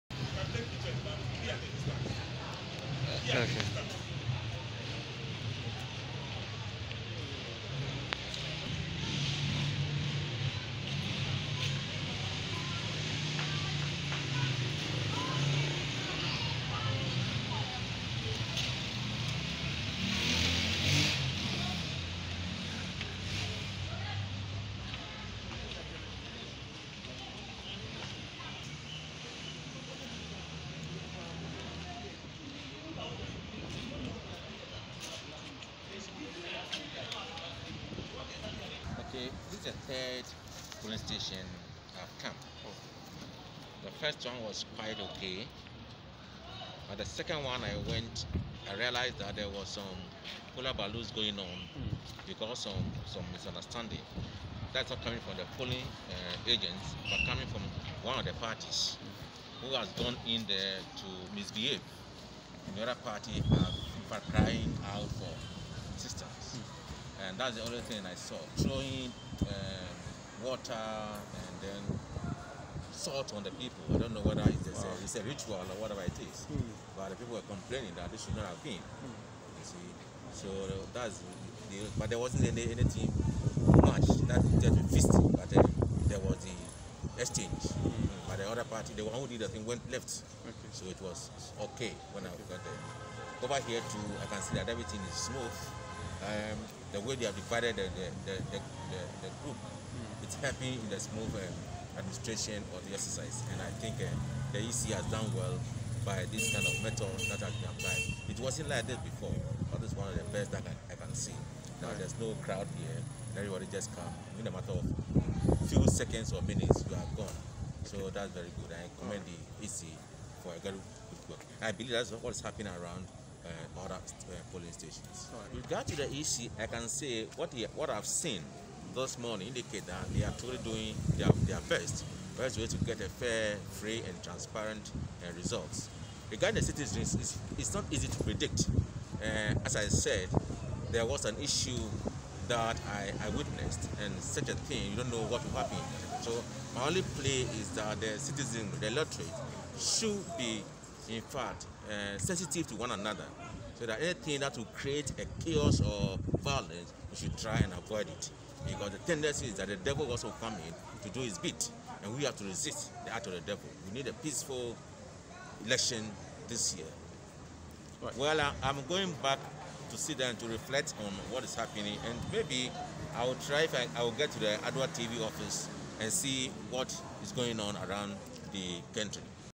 RT. REV . DR S.M TORTO CASTS HIS VOTE
The Anglican Bishop of Accra has exercised his civic responsibility at Holy Trinity Senior High School